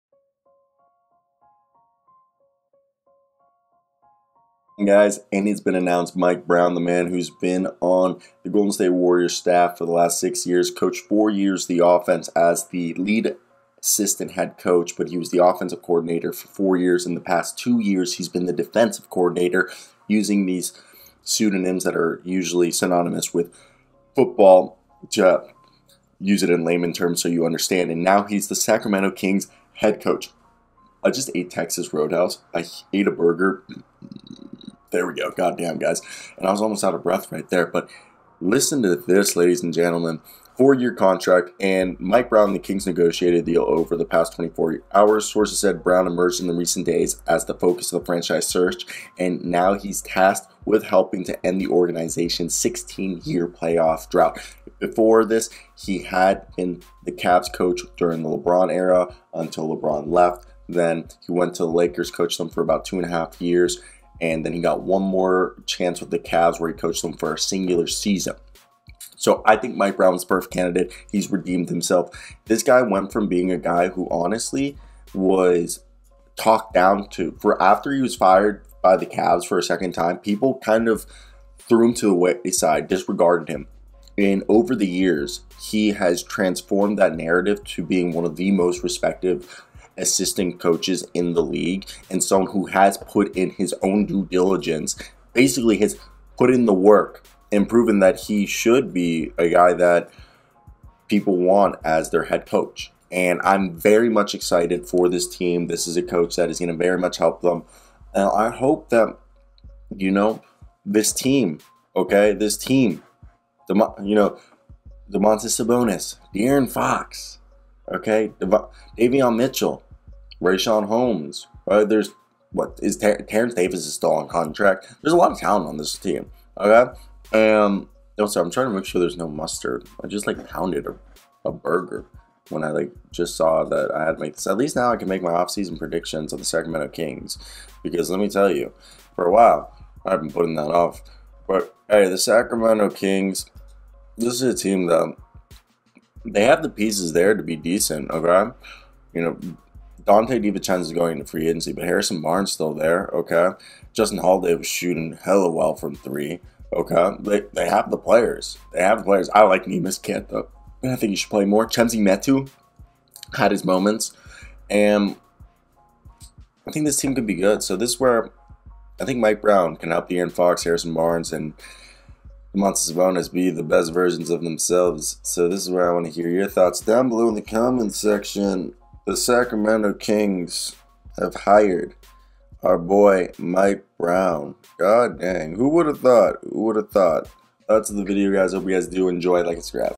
The Sacramento Kings Hire Mike Brown As Their Head Coach
The Sacramento Kings have agreed with Golden State assistant Mike Brown on a four-year contract to become the franchise’s next coach, sources tell ESPN. Mike Brown and Kings negotiated deal over the past 24 hours, sources said. Brown emerged in recent days as the focus of franchise’s search and now he’s tasked with helping to end the organization’s 16-year playoff drought.